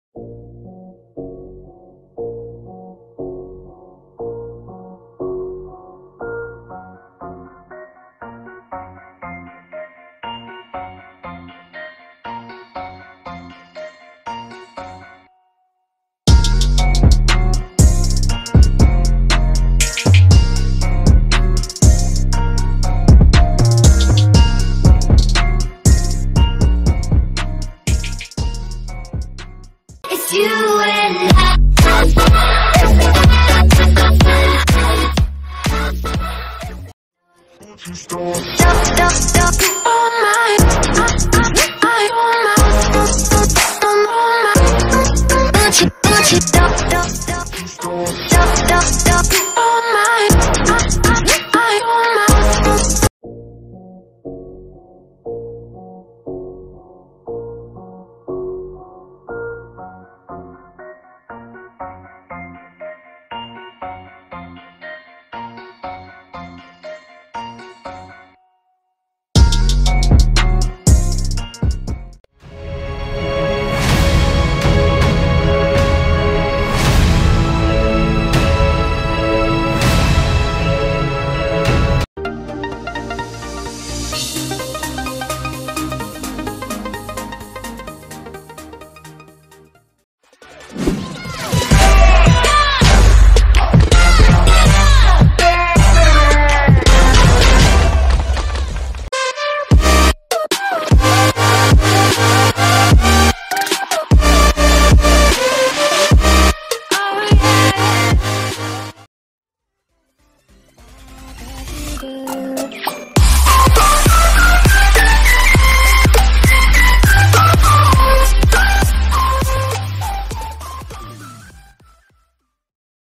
Intro of our you-tube channel by filmora
This video is sample of intro made by fimora channel. All viewer can also get some ideas with this video for creating intro of your channel.